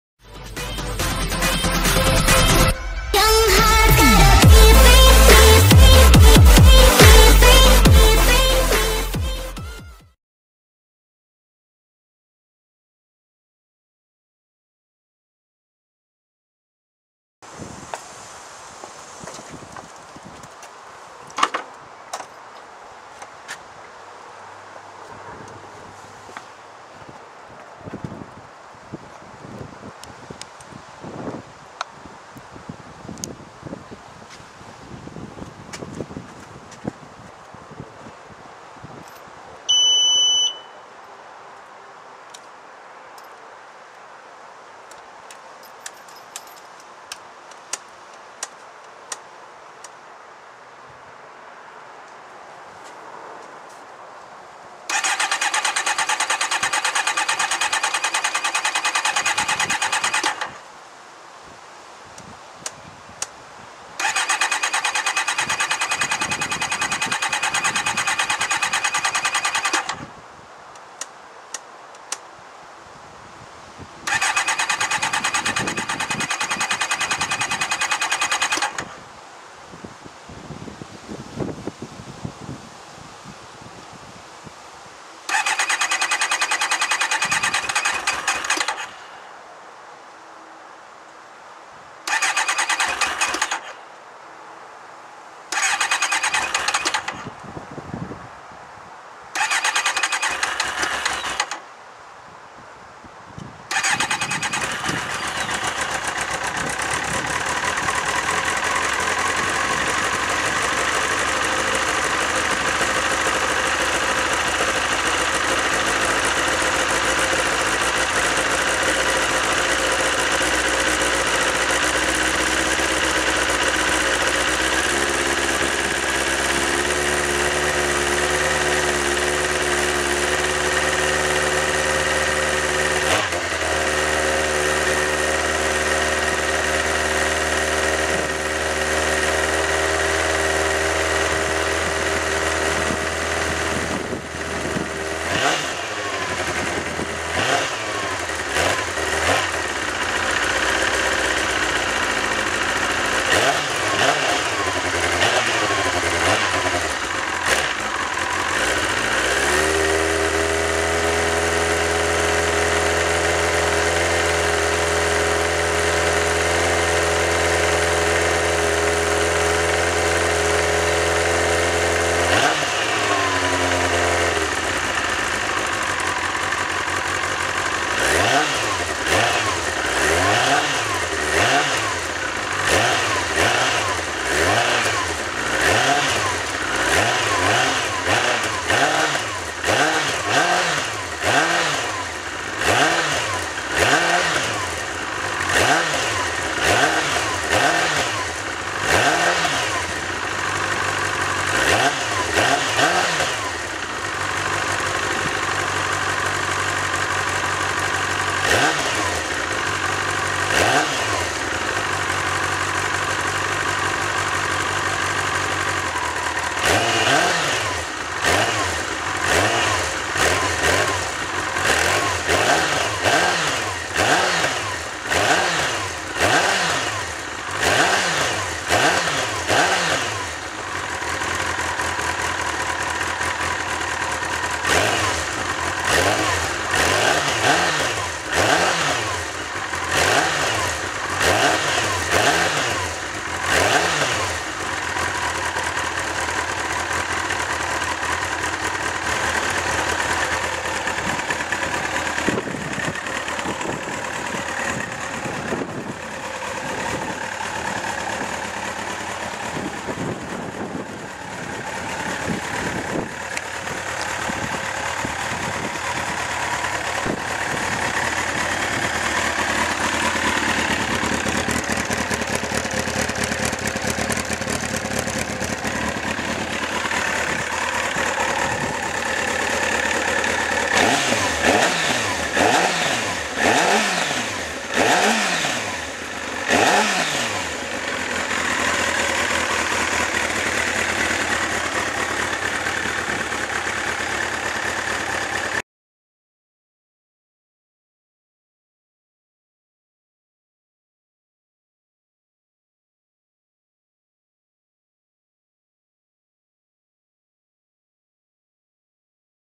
Kawasaki ZX9R Ninja First (difficult) Start in around 6 months...
The poor old girl My 1998 Kawasaki ZX9R Ninja has sat in the garage far too much this year...maybe we will get some good days to see out the rest of the year, And ill get time to enjoy her !!!...always found the ZX9R a great bike and although I'm getting too old for a sports bike, i cant see me ever getting rid of her.